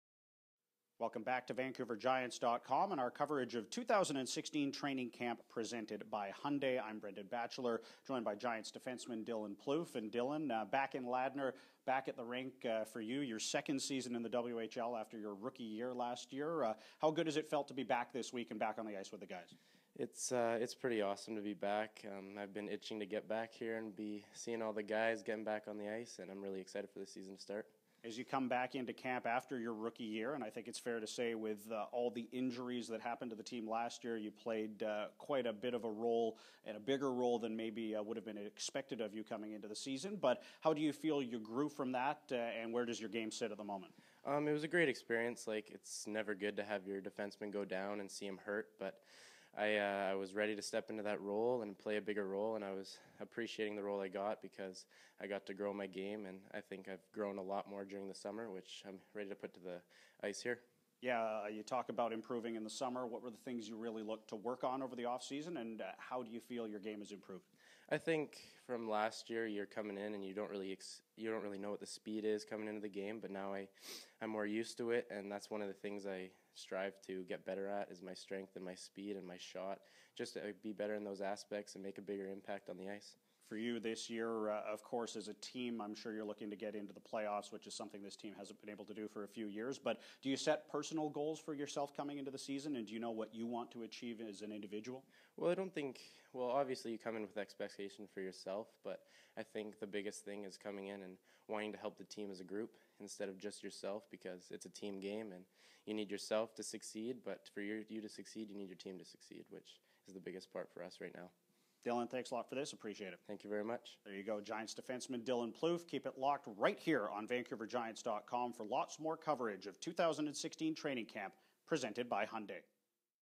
2016 Training Camp Interview - Dylan Plouffe
Brendan Batchelor talks to Giants defenceman Dylan Plouffe at 2016 Vancouver Giants Training Camp, presented by Hyundai.